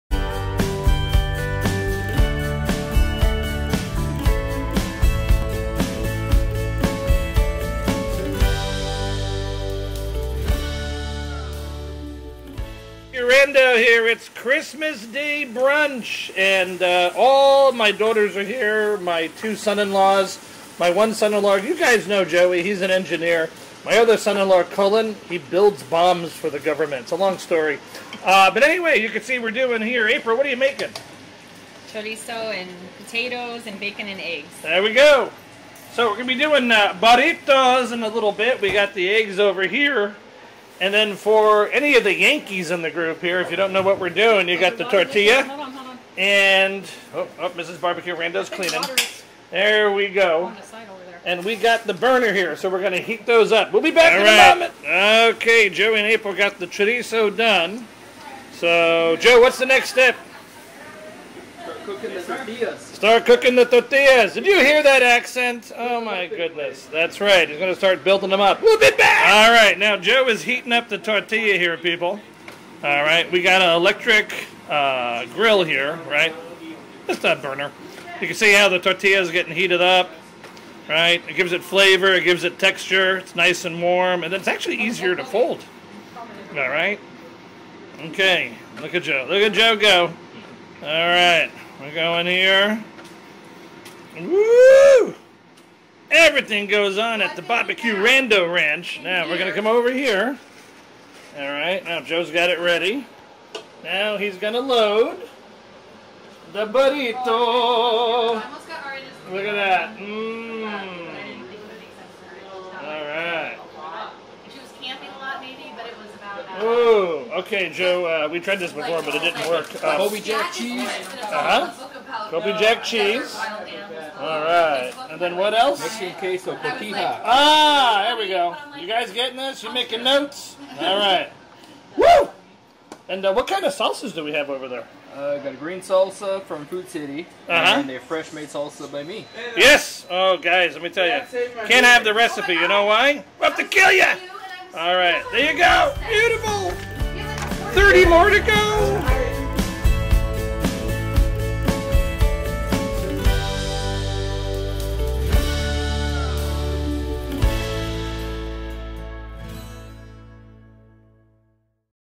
Christmas Brunch 2016 @ BBQ Rando's Family Ranch
Merry Christmas from the BBQ Randu Family Ranch! We celebrated Christmas Day with a family brunch and it was done BBQ Rando style!

We wish you the best Merry Christmas ever! Family comes first!

Proud Member of Pyro Nation
Live like you are on Vacation!